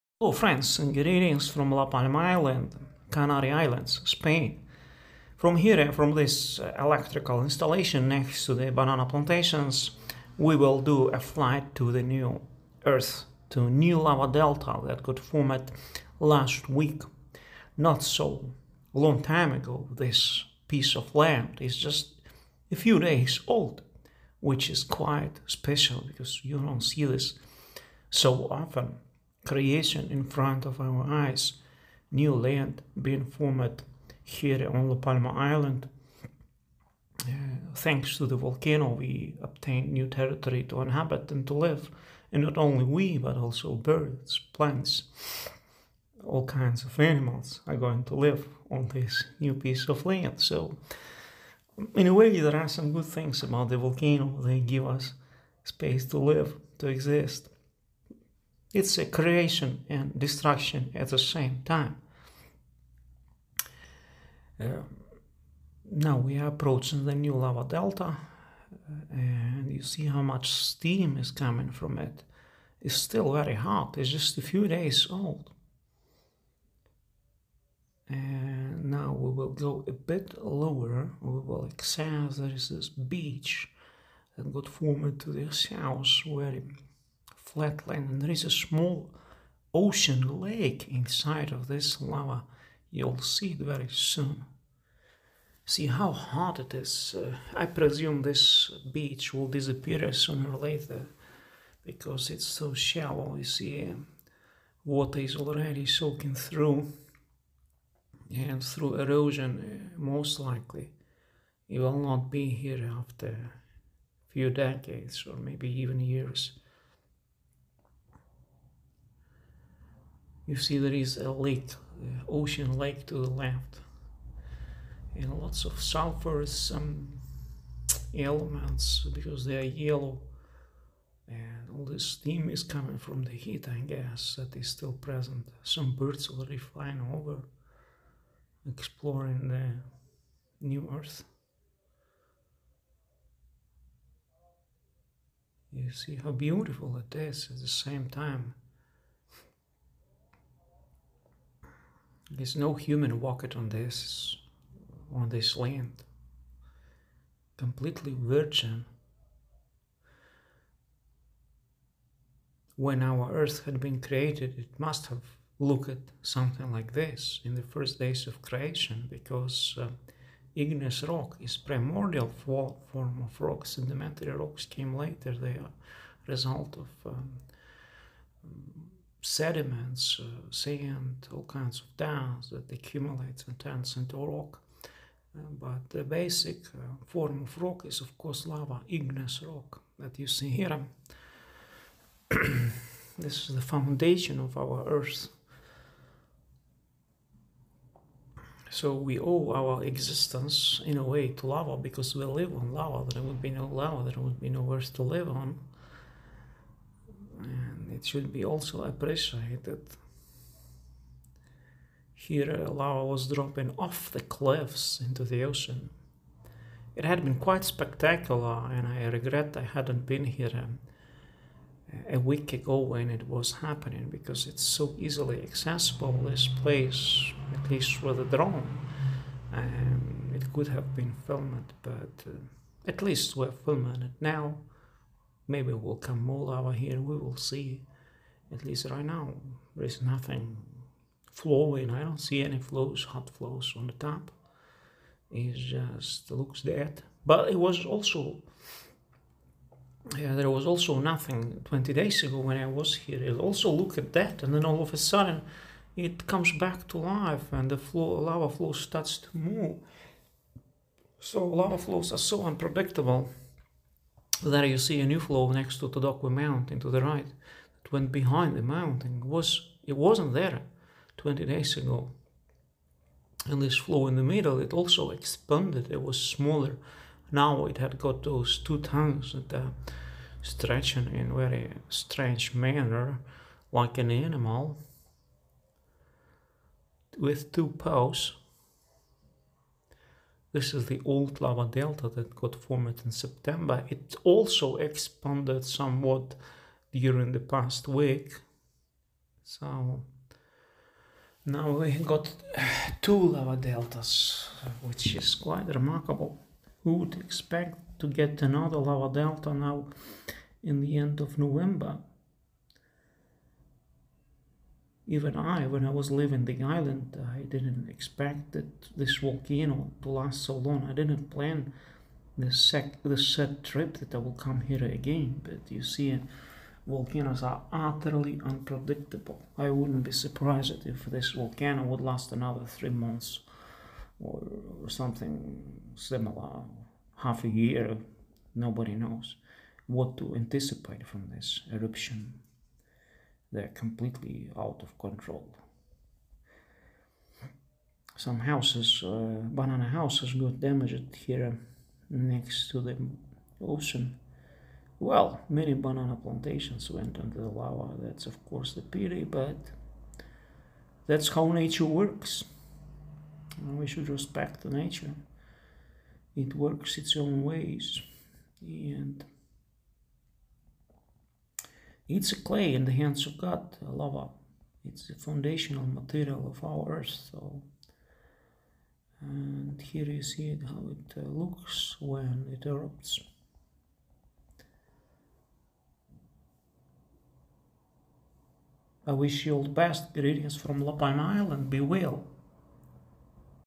La Palma Drone: The newest lava delta to the north! Lava entered ocean here last week! 30.11.21 4K
Exploring the newest earth. New lava delta ro the north. 4K Drone.